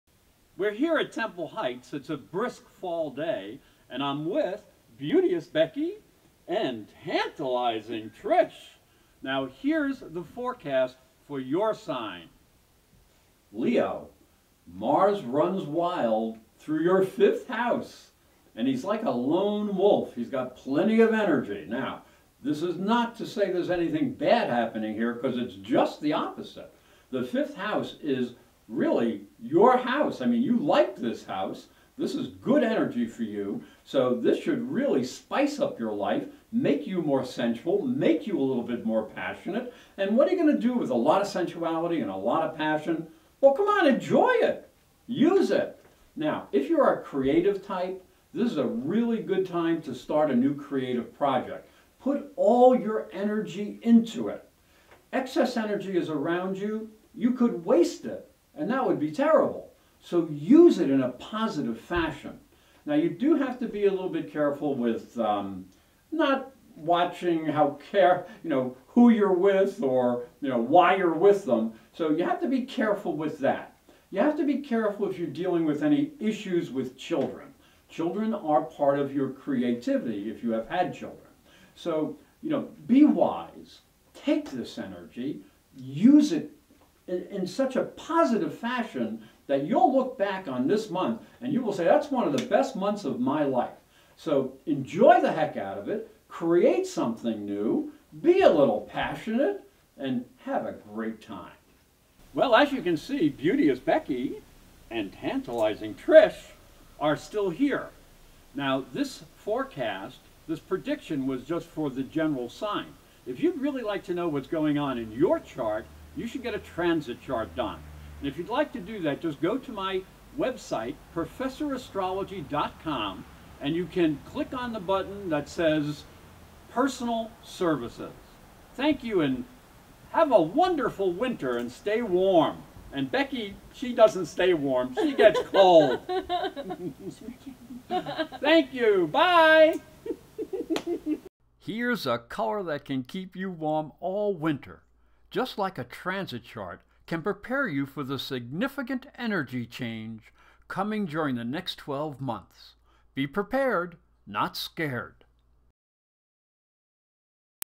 Leo Horoscope November 2010
Leo Horoscope for November 2010 Horoscope for November 2010 with Tantalizing Trinsh and Beauteous Becky produced at Temple Heights Spiritualist Camp in Northport Maine